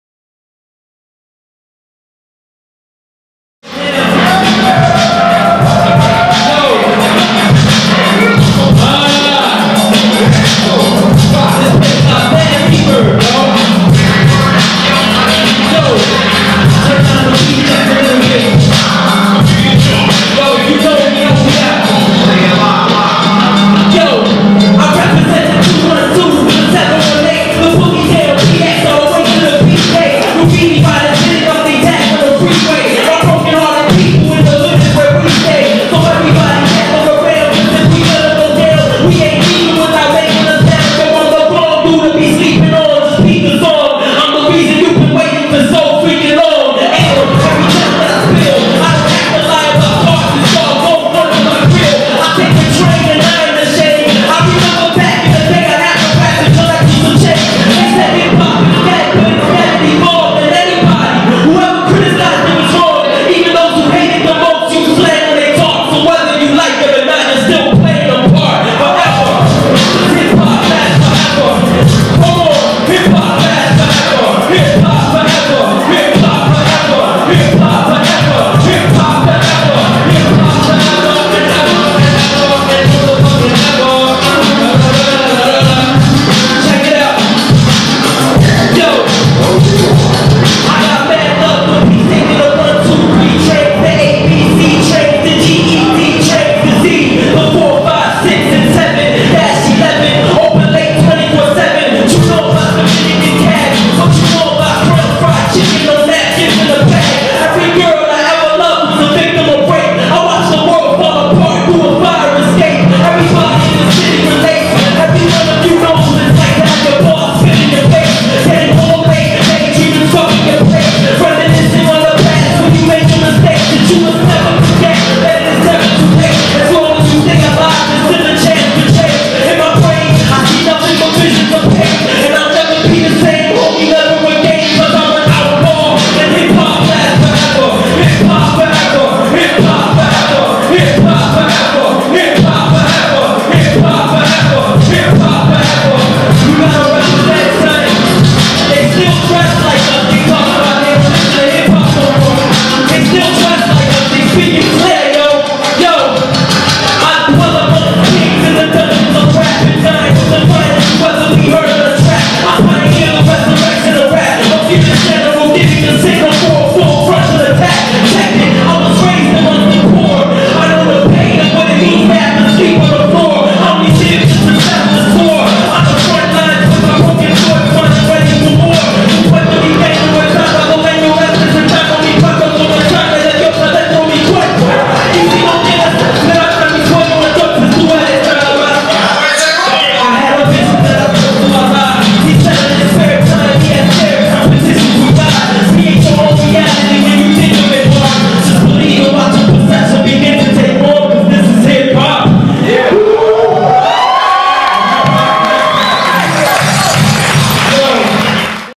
The Art of Lyrics presents Final Outlaw - AOL #7 112009
The Art of Lyrics #7 - Hosted by MC Rhino & Tony Stanz.  Resident DJ FREDONE & Polarities.  Featuring Breeze Evah Flowin' with Kid Lucky, Savvy, Percepto, Prime Prolifik, Rugged & Raw, Hasan  Salaam, & Final Outlaw.